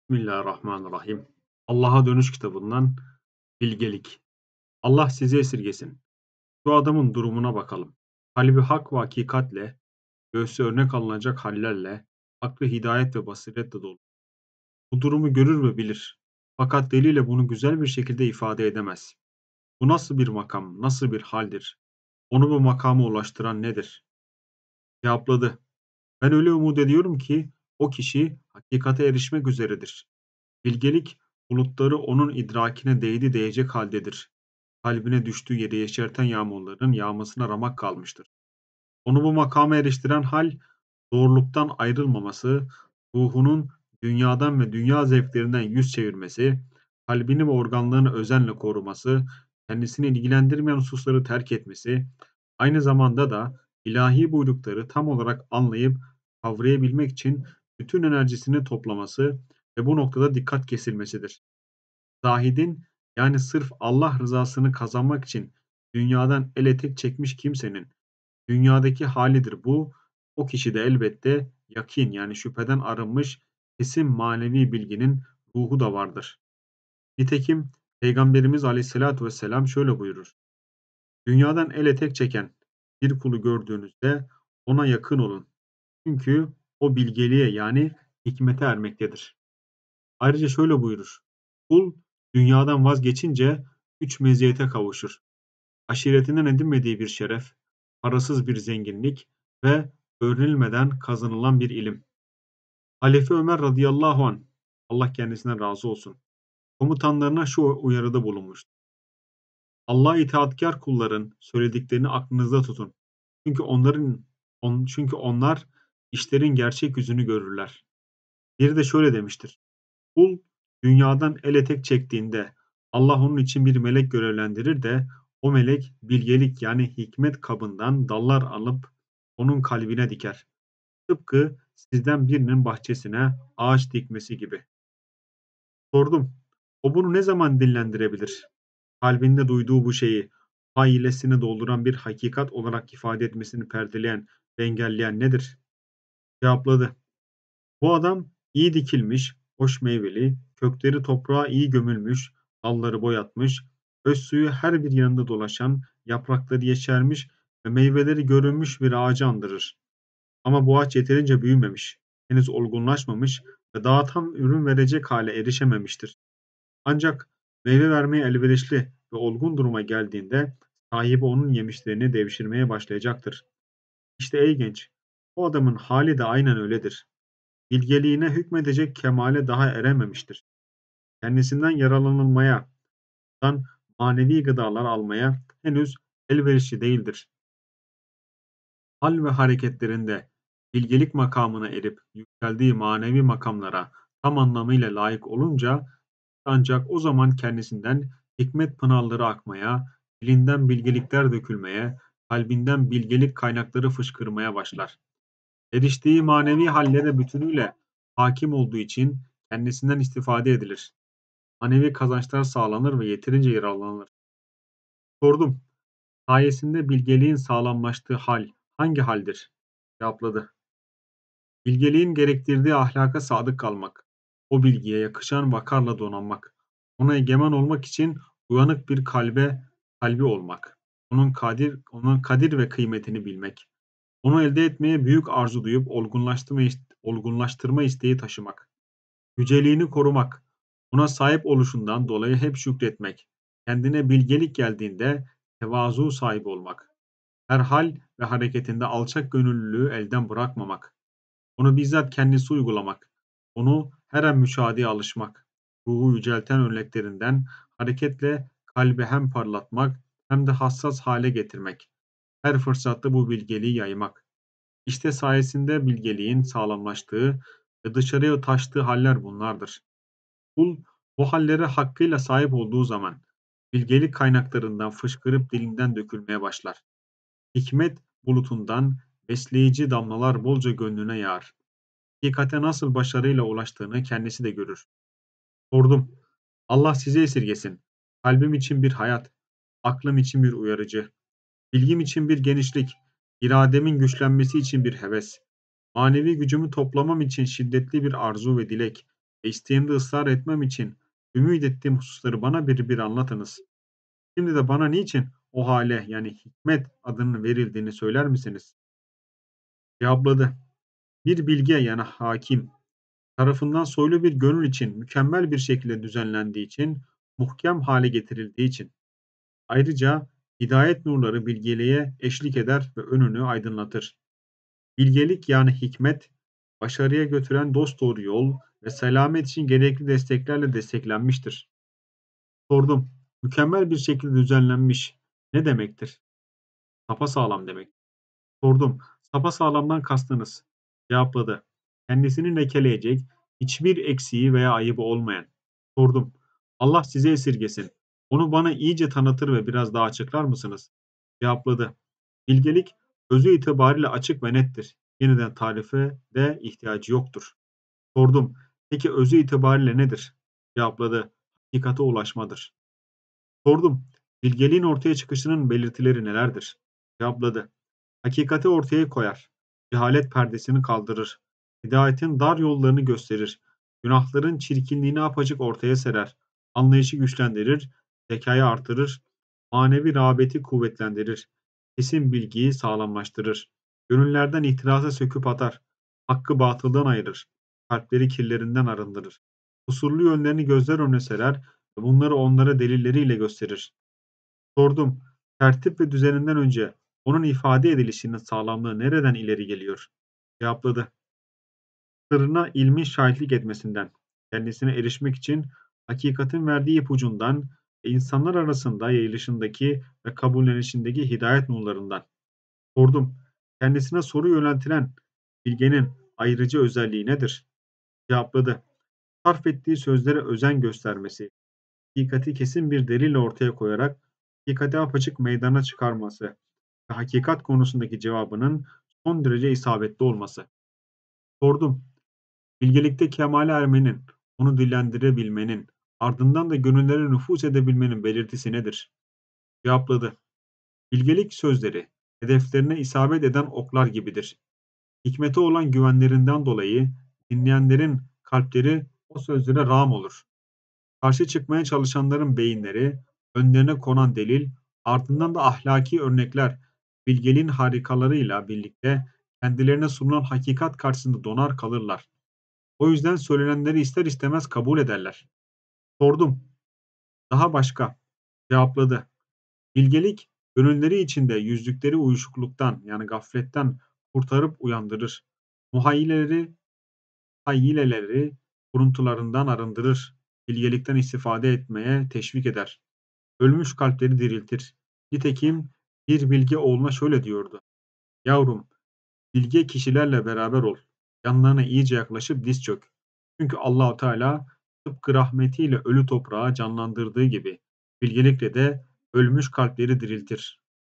Hikmet (Bilgelik) - Allah'a Dönüş - Hâris el-Muhâsibî
Büyük ruhaniyetli âlim Hâris el-Muhâsibî, Hasan-ı Basrî hazretlerinin talebesi Cüneyd-i Bağdadî başta olmak üzere birçok ünlü sûfînin hocasıdır. Abdülkâdir Geylânî ve İmam Gazzâlî gibi maneviyat üstadlarının da ilham aldıkları seçkin bir mutasavvıftır.

Bütün İslâmî ilimlerde zamanının en bilgili kişisi olarak kabul edilmiş, ayrıca kendisi ilim çevrelerince de âlimlerin ve sûfîlerin kutbu olarak görülmüştür.

İnsan psikolojisini en iyi bilen, nefsin aldatmacalarını ve ruhun hastalıklarını çok iyi teşhis eden ve nasıl tedavi edileceği konusunda son derece yetkin olan Muhâsibî, bu eserinde insanın Allah ile bağını tekrar nasıl kurabileceğinin ve insanın nasıl kendisiyle barışık hâle gelip ruh huzuruna erebileceğinin yolunu gösteriyor.